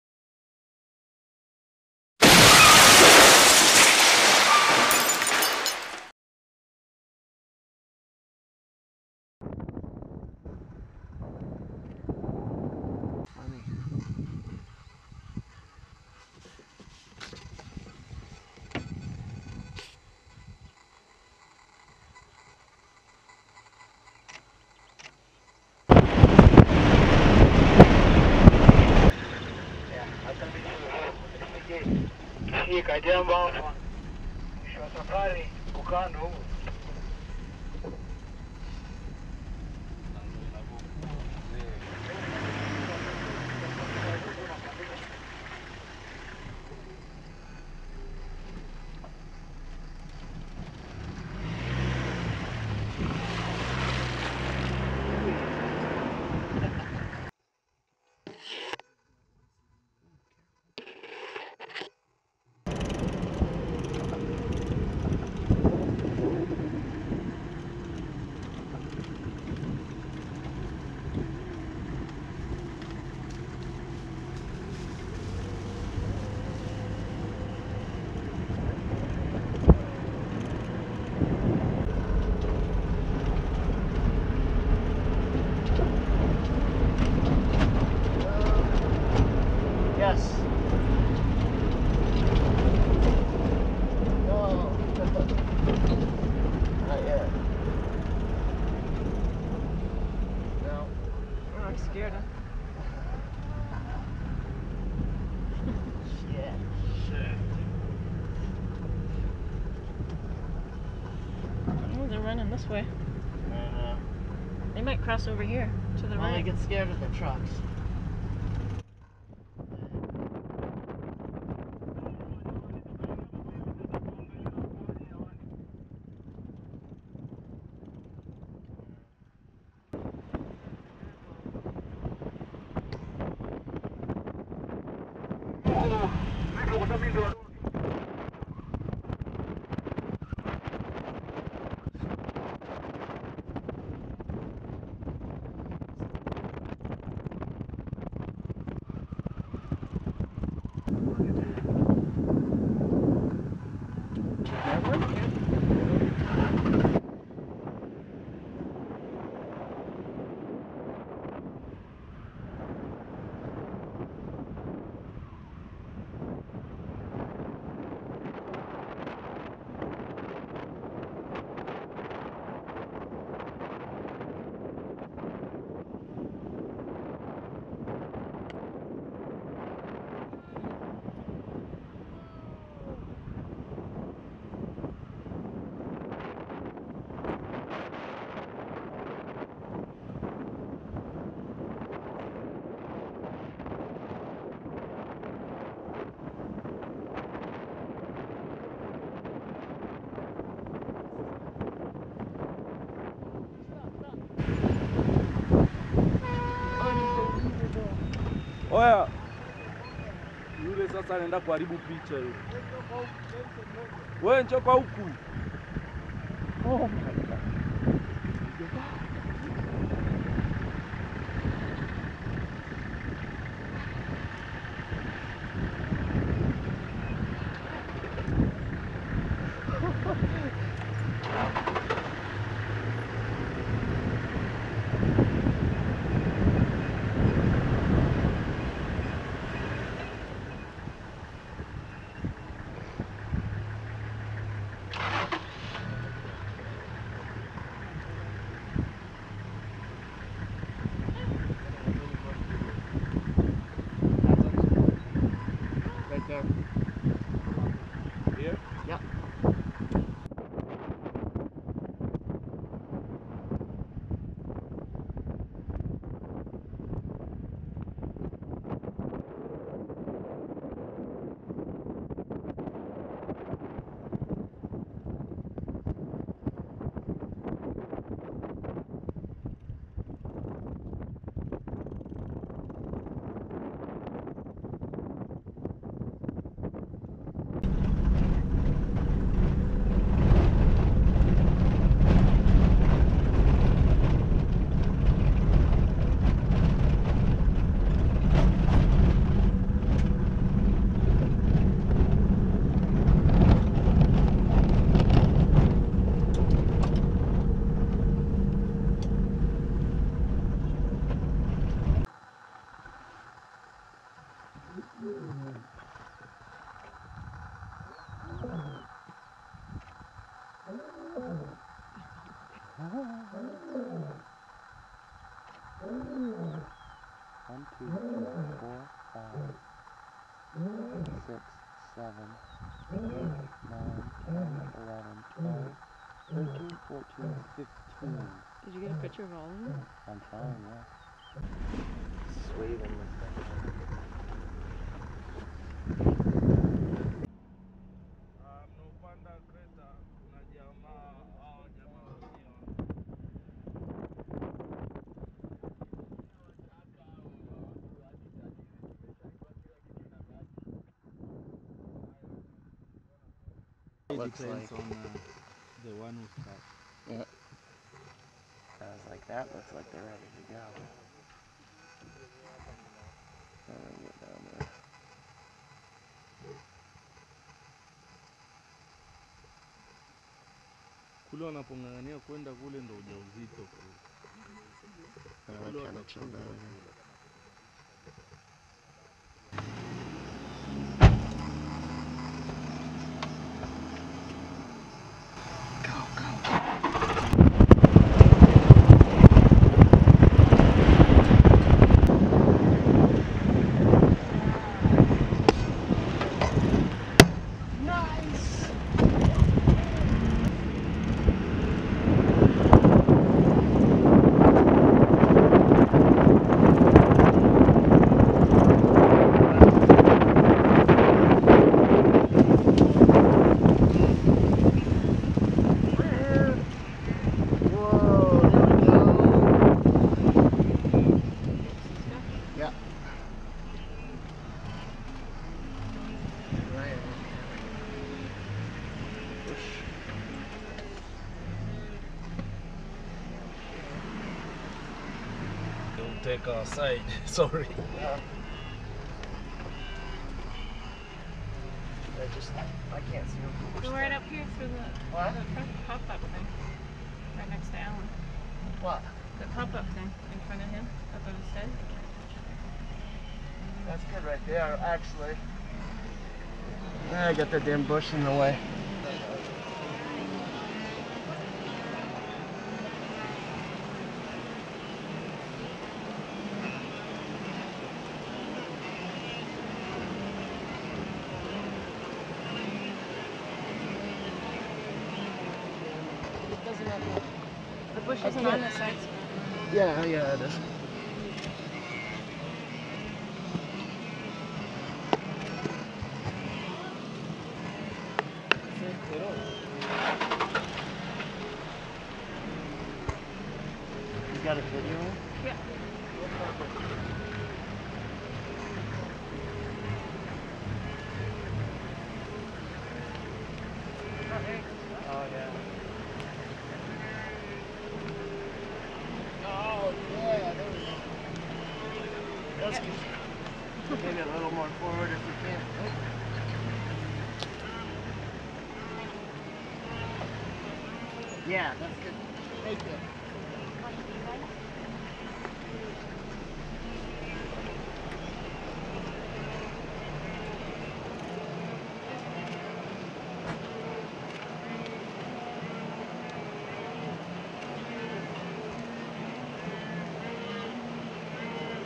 Behind the Scenes of the Africa Safari Shoot 4k UHD
Behind the scenes shots of the Africa Safari filmed by Tracy Dunne. Some fun footage showing the challenges and adventure of shooting in the Serengeti.